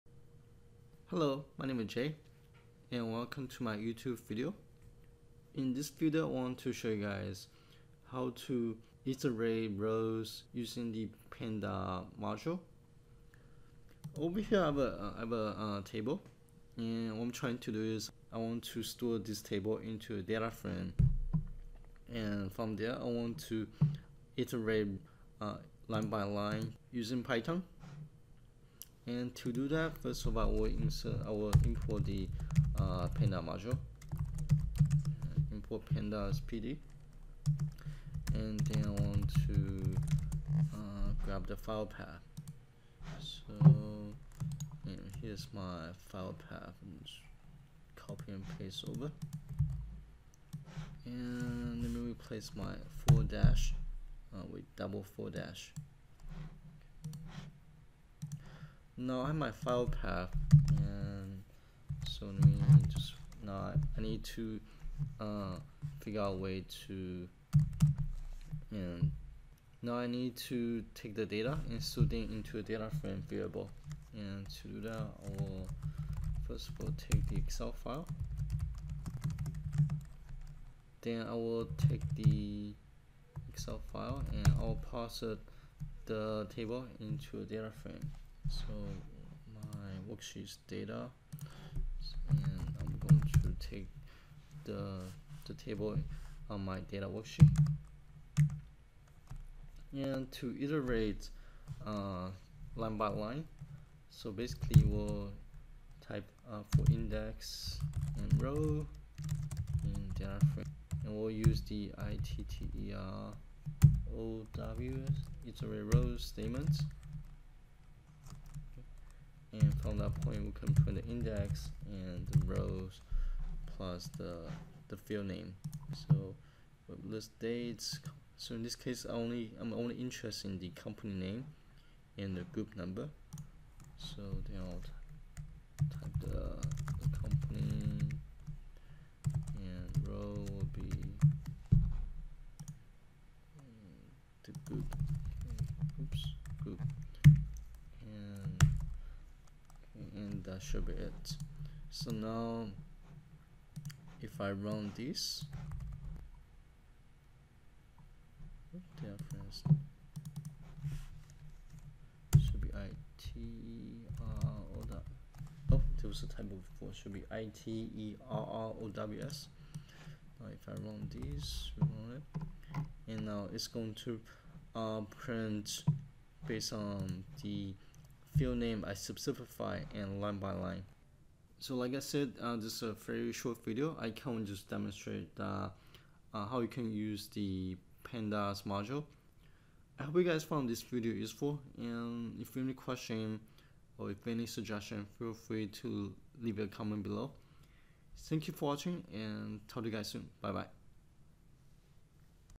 How to Iterate Over Each Rows in a DataFrame in Python (pandas)
How to use the pandas module to iterate each rows in Python. This is a common question I see on the forum and I thought I make a short video demonstrate how to do that.